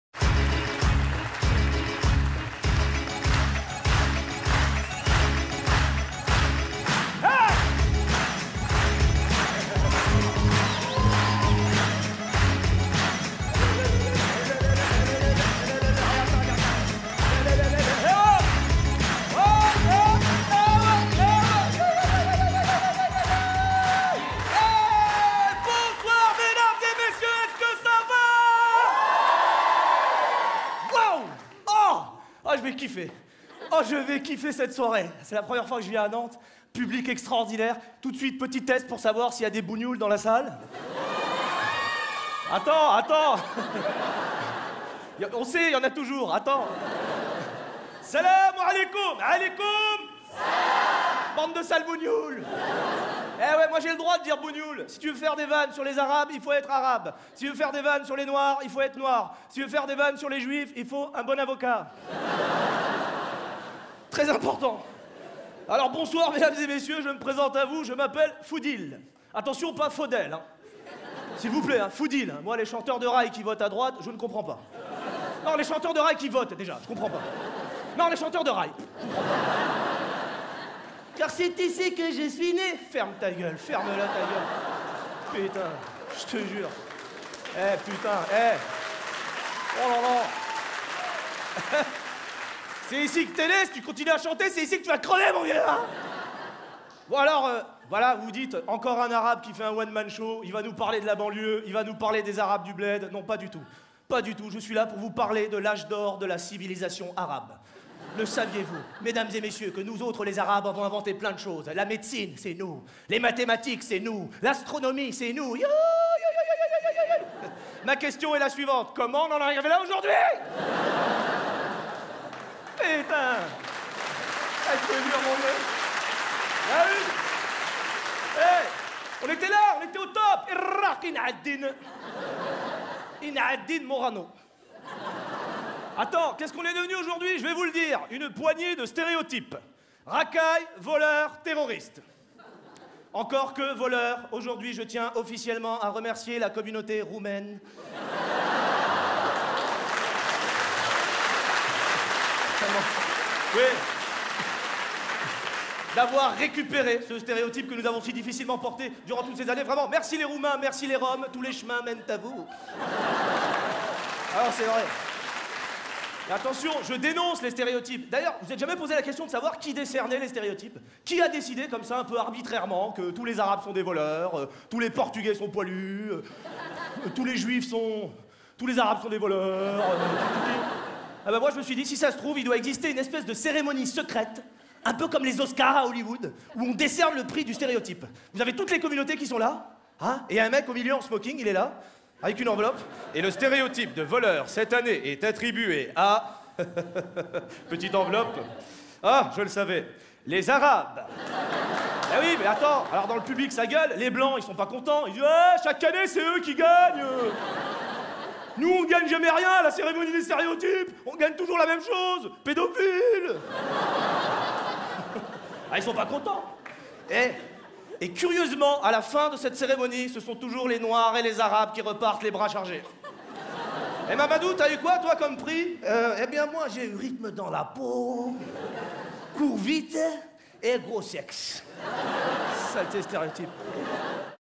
FOUDIL KAIBOU - L'intégration par le vin
Foudil Kaibou au Comedy Club jusqu'au 30/07/2015

Sketch de Foudil Kaibou au sujet de l'intégration des étrangers en France.

Foudil Kaibou nous parle des préjugés dans cette vidéo garantie sans idées reçues.

Auteur et Interprète : Foudil KAIBOU
Réalisateur : Christophe FRANCK
Titre du sketch: L'intégration par le vin - extrait 1